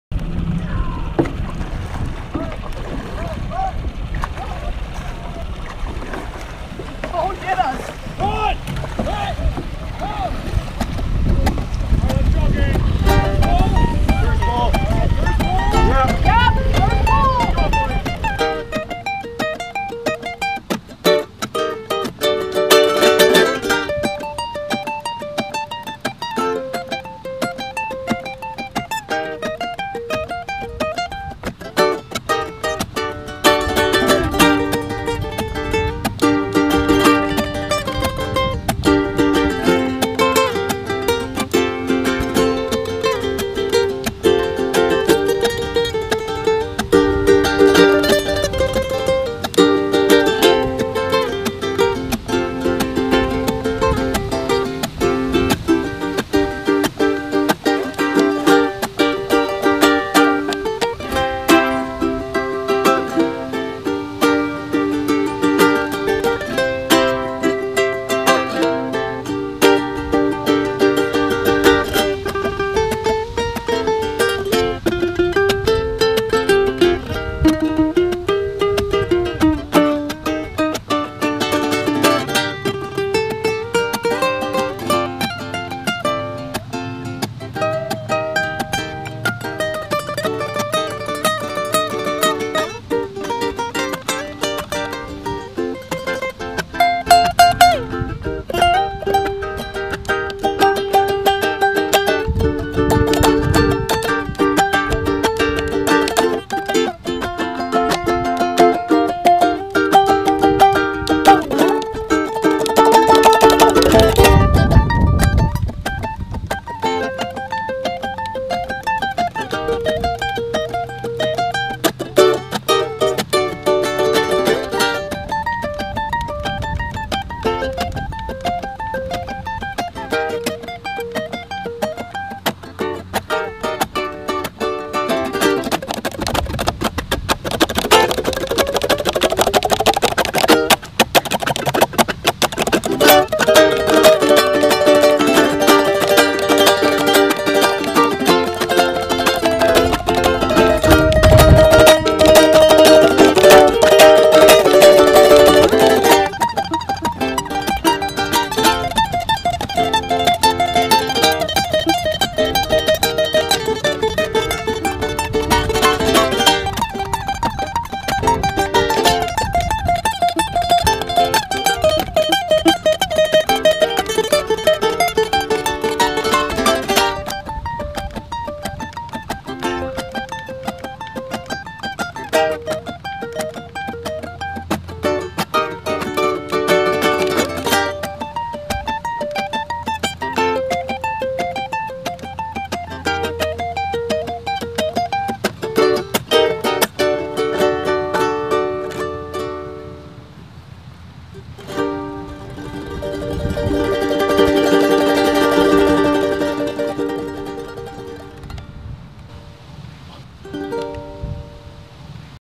Mid Pacific Paddling
The 2016-17 paddling season filmed and edited by Mid Pacific's Robotics Drone Technology Division